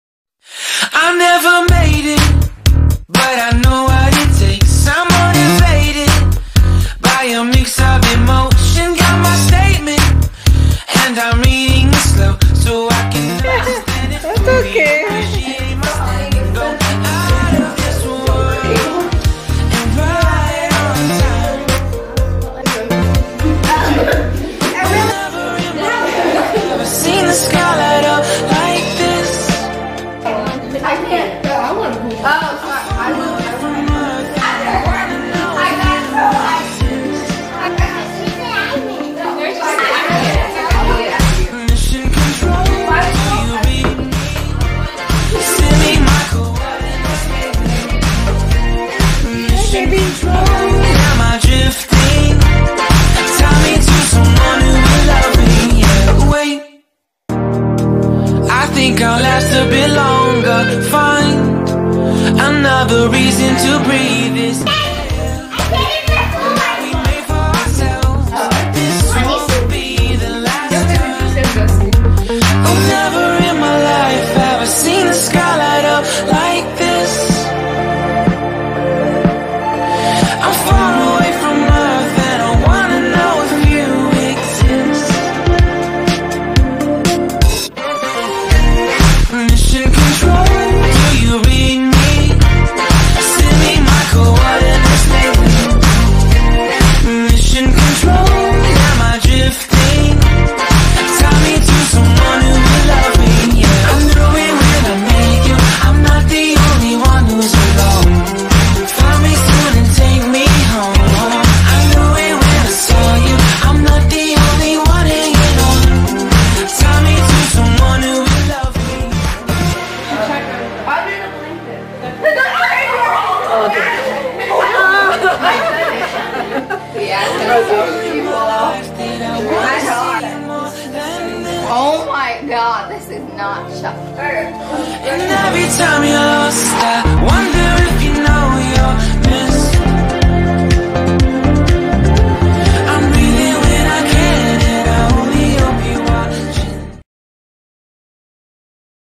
Rashmi Ashok - Summer Sleepover 2019 With Friends !!
Rashmi having a wonderful time with her friends - Summer 2019